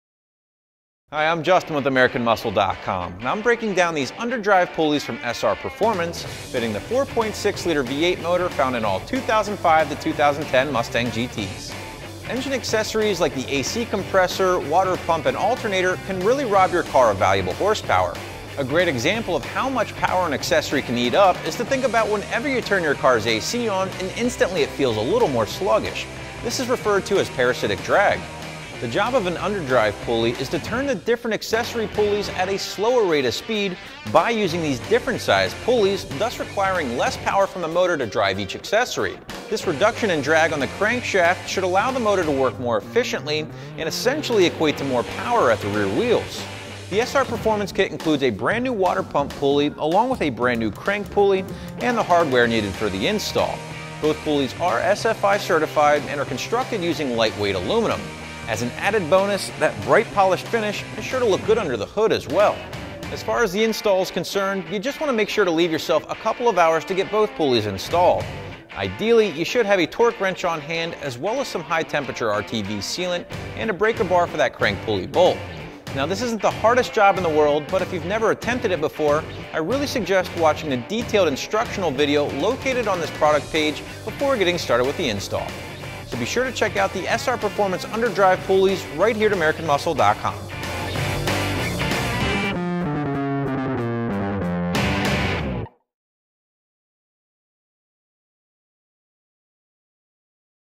2005-2010 Mustang GT SR Performance Underdrive Pulleys Review & Install
Mustang Fitment: 2005, 2006, 2007, 2008, 2009, 2010

Unleash 15 More Horsepower. These SR Performance Underdrive Pulleys can unleash more horsepower from your 4.6-liter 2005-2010 Mustang. You can expect to enjoy up to 15 horsepower more by simply bolting on these high-quality aluminum underdrive pulleys. Various accessories such as the A/C compressor and the water pump will rob your motor of some much-needed power. These SR Performance Underdrive Pulleys will give your pony car more horsepower by turning each pulley at different speeds, thereby reducing the parasitic drag on your high-performance motor. Made from the finest quality aluminum material, the shiny finish will also add a dose of sporty style under the hood. The kit includes the water pump pulley and the crank pulley.
Made from High-Quality Aluminum.  These lightweight underdrive pulleys are CNC-machined from high-quality aluminum for an exacting fit. Each piece is polished for lasting brightness and a cleaner aftermarket look.
Professional Installation Required.  If you know your way around cars then the install will only take you an hour or two. You will also need tools such as a torque wrench, breaker bar, and some high-temp RTV sealant.  Each piece is precision crafted to fit on the 2005 to 2010 Mustang, so you don’t need to modify anything to install this kit. Simply remove the old pulleys and bolt in the SR Performance Underdrive Pulleys.